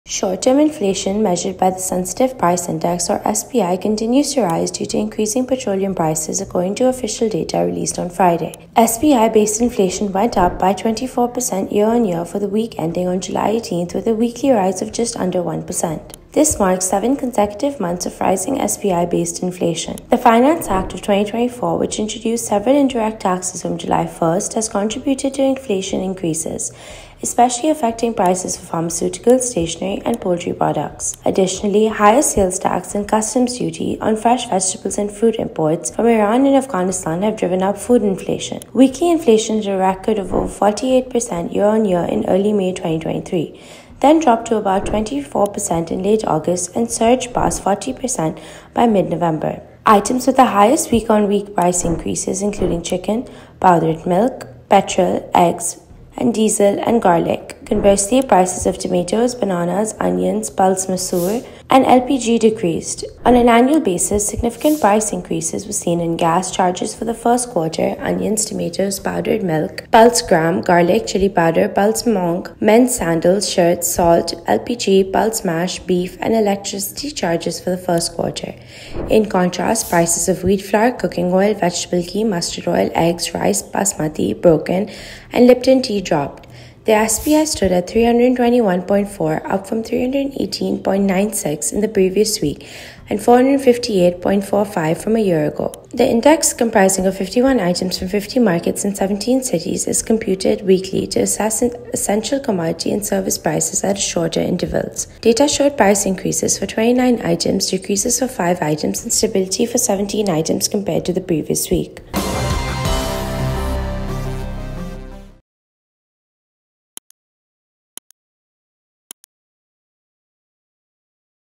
Expensive Fuel Keeps weekly inflation Elevated in Pakistan | Dawn News English
Short-term inflation, measured by the Sensitive Price Index or SPI, continues to rise due to increasing petroleum prices, according to official data released on Friday. SPI-based inflation went up by about 24 percent year-on-year for the week ending July 18, with a weekly rise of just under one percent